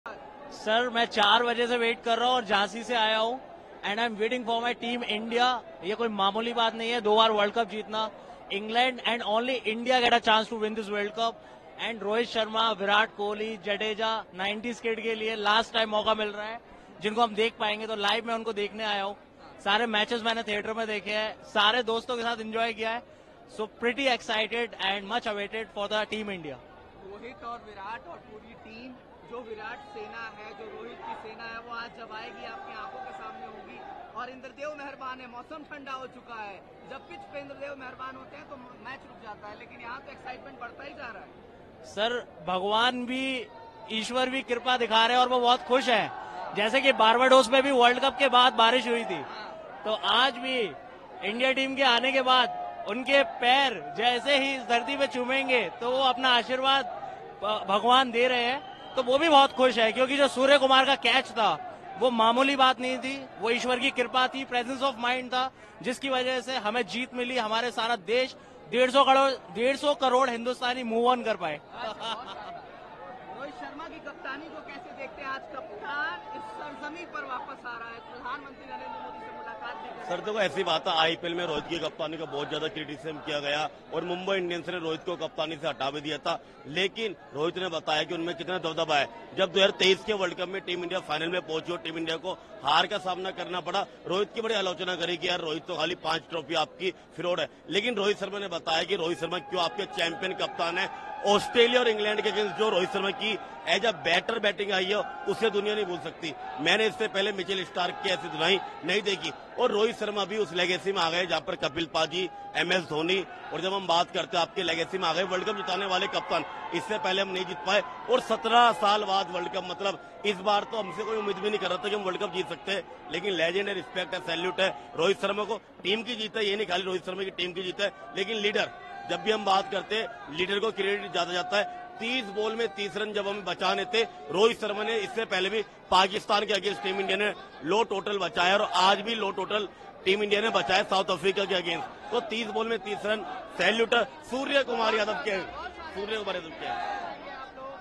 Team India Arrival | ৪বজাৰ পৰা প্ৰিয় খেলুৱৈক ৰৈ ৰৈ স্বাগতম অনুৰাগীৰ। N18V
Team India Arrival | পুৱা ৪বজাৰ পৰা প্ৰিয় খেলুৱৈক ৰৈ ৰৈ স্বাগতম অনুৰাগীৰ।